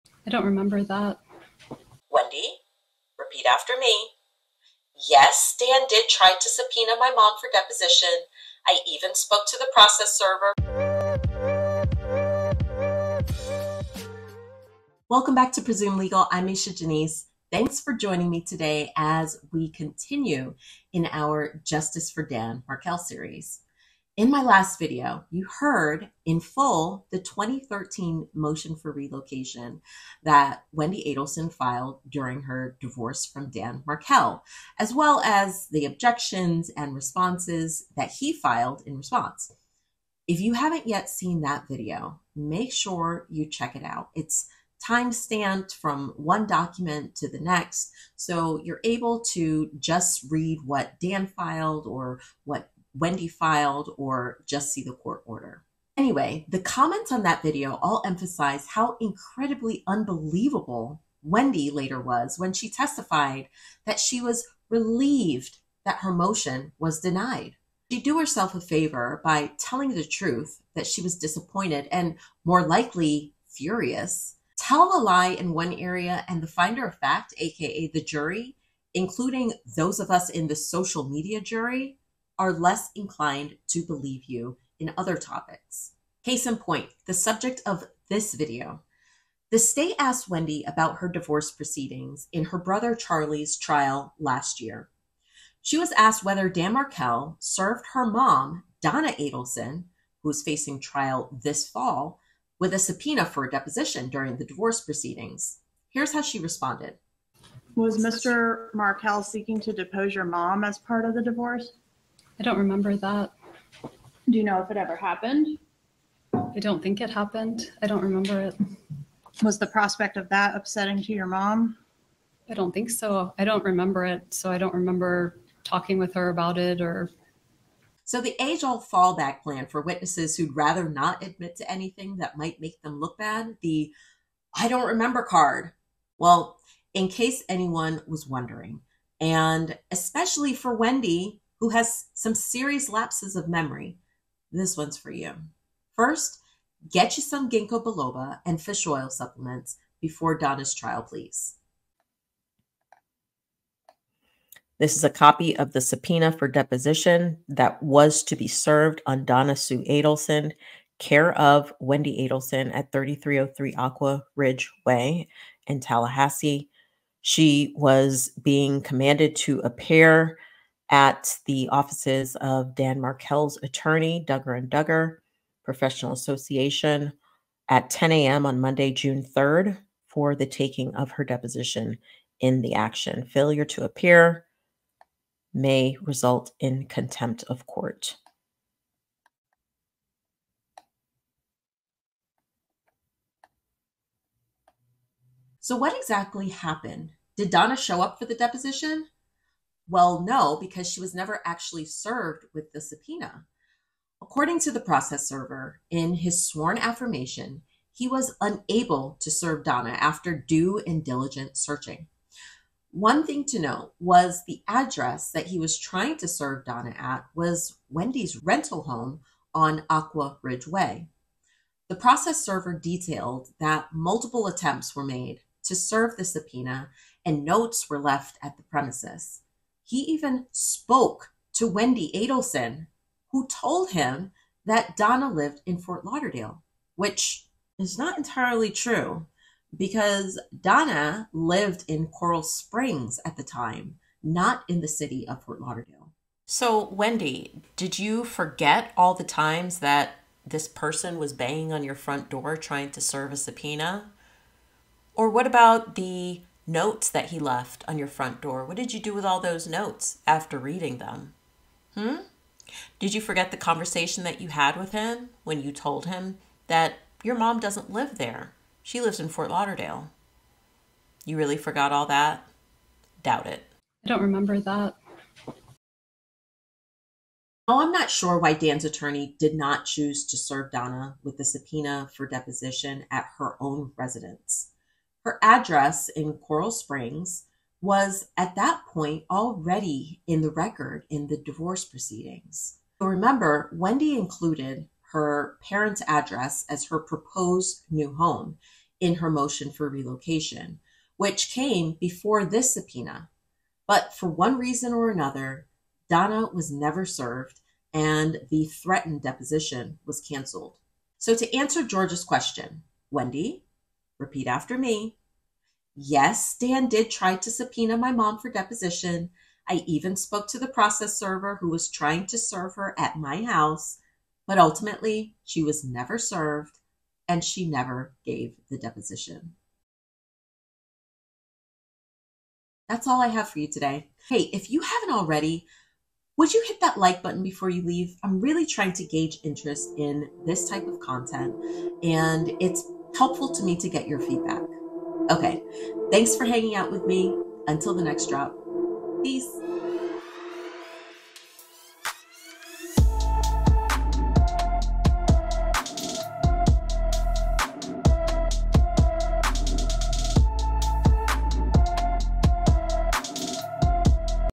Donna’s Deposition Subpoena in Wendi’s Divorce
IN THIS VIDEO, I remind Wendi of the time during her divorce from Dan Markel when he tried, unsuccessfully, to serve a subpoena for deposition on her mom, Donna.
Markel was murdered in 2014 in a hit operation ordered by Wendi’s brother, Charlie Adelson, who was convicted in 2023. Wendi’s mother, Donna Adelson, was arrested after Charlie’s conviction at the airport attempting to flee on a one-way flight to a non-extradition country. Donna is charged for her role in Dan Markel’s murder and will be tried in Leon County, FL in 2024.